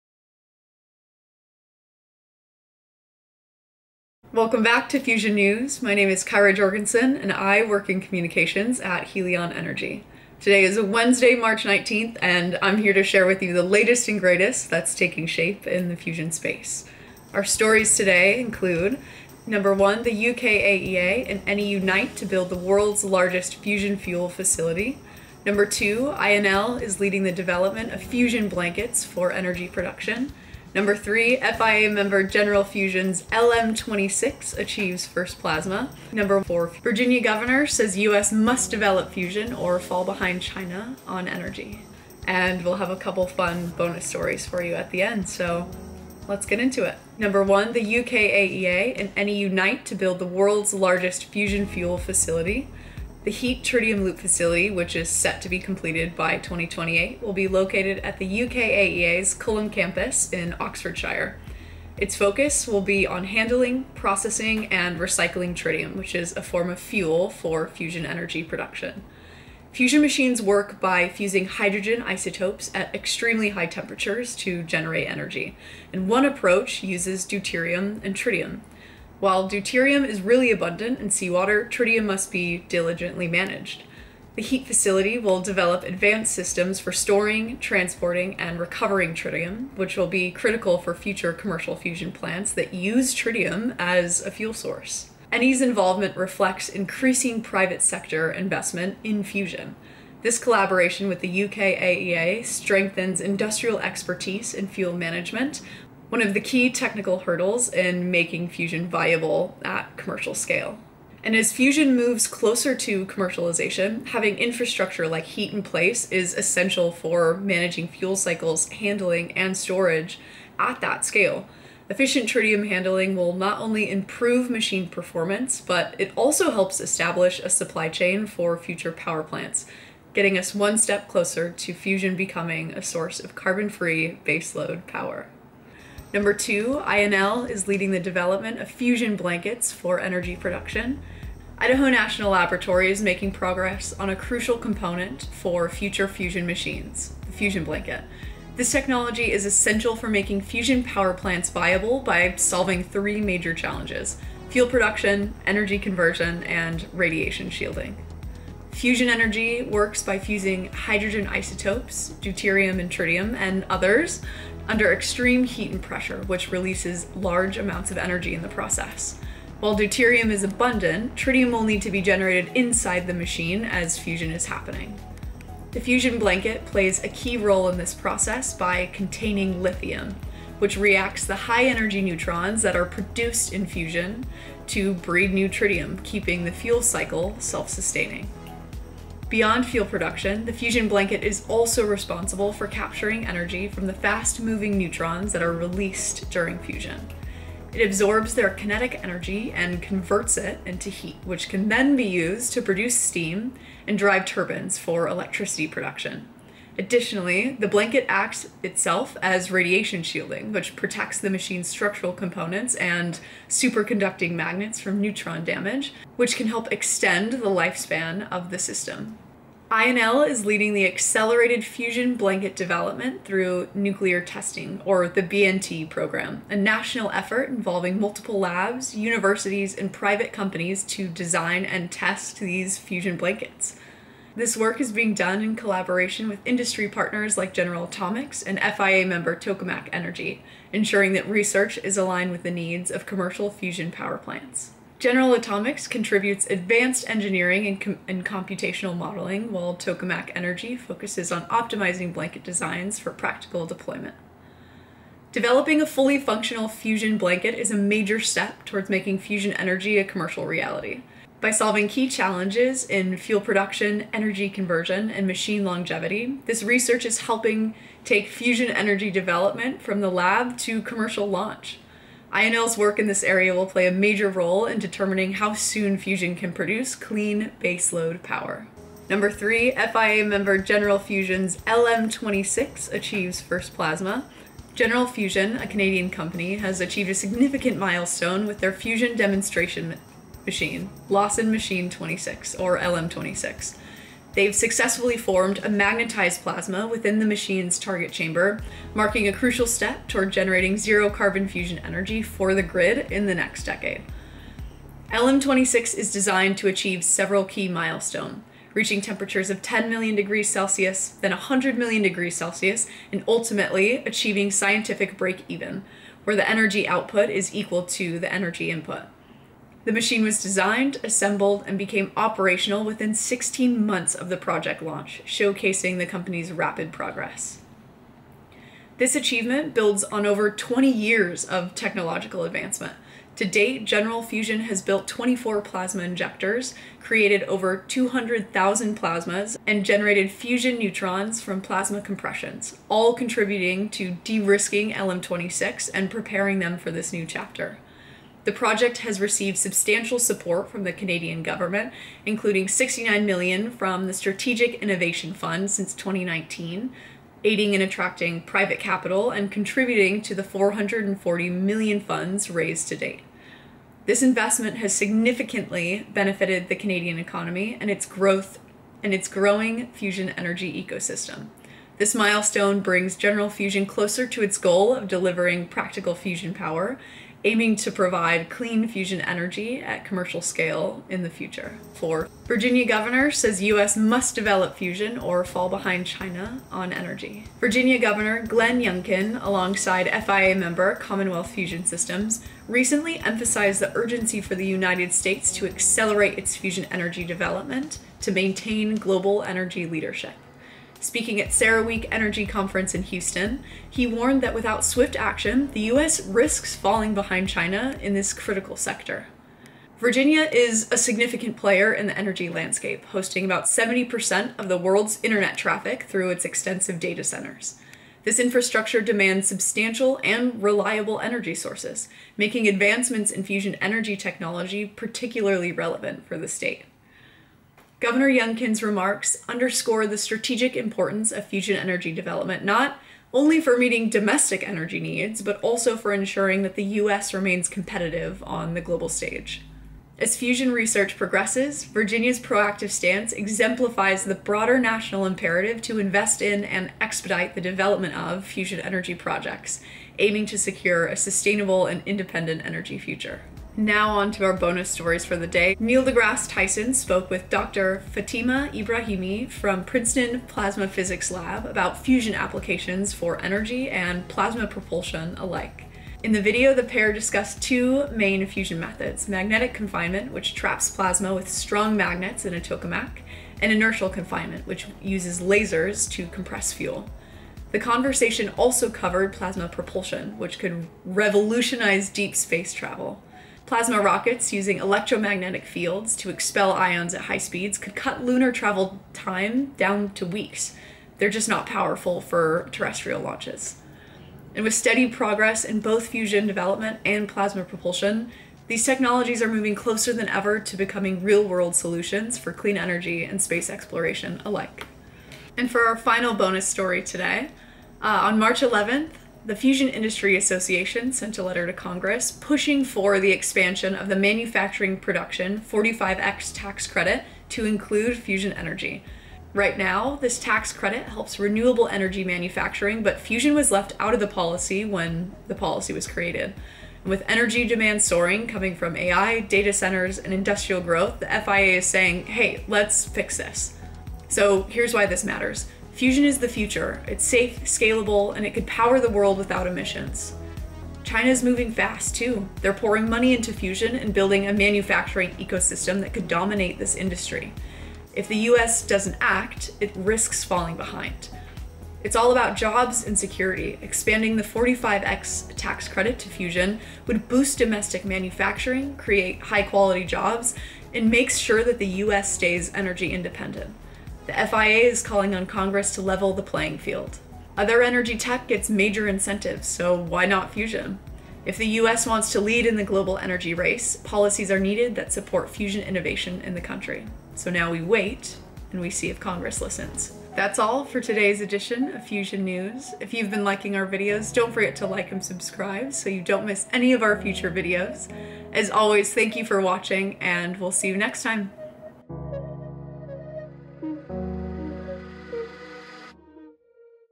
Fusion News, March 19 2025
Kyra Jorgensen, working at FIA member Helion Energy, gives today's global fusion news update. Links to all of the stories mentioned are included below.

1.UKAEA and Eni Unite to Construct the World's Largest Fusion Fuel Facility

2. US to test crucial nuclear ‘fusion blanket’ to produce sun-like power on Earth

3. General Fusion’s Made-in-Canada Technology Achieves First Plasma in Cutting Edge Fusion Demonstration Machine

4. Virginia governor say US must develop fusion or fall behind China on energy